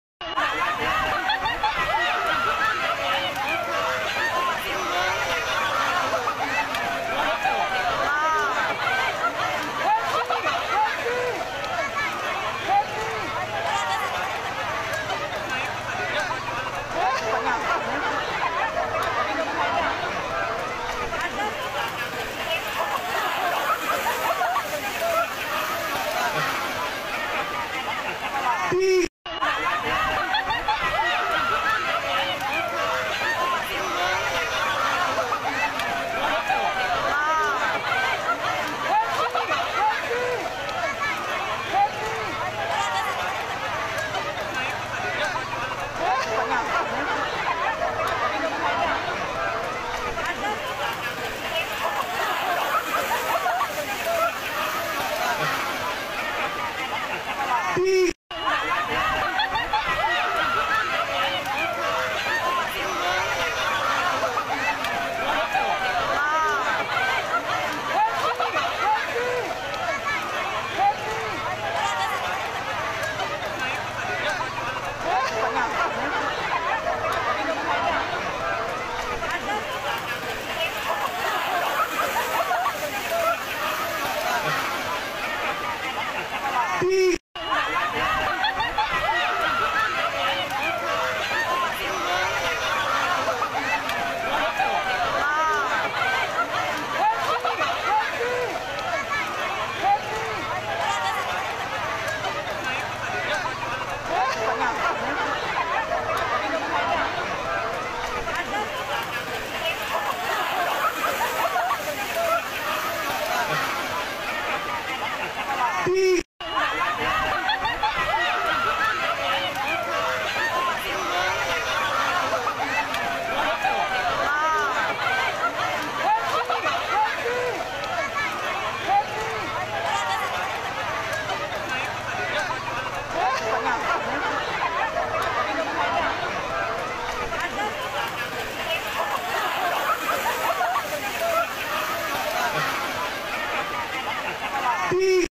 Pertarungan berani sakit, VIRAL PERANG KETUPAT DI TEMPILANG 2024
Salah satu Tradisi adat bangka yg dk pernah luntur pradik ok.
Perang ktupat hari ni tanggal 3 maret 2024 di pantai pasir kuning kec tempilang bangka barat. provinsi Bangka Belitung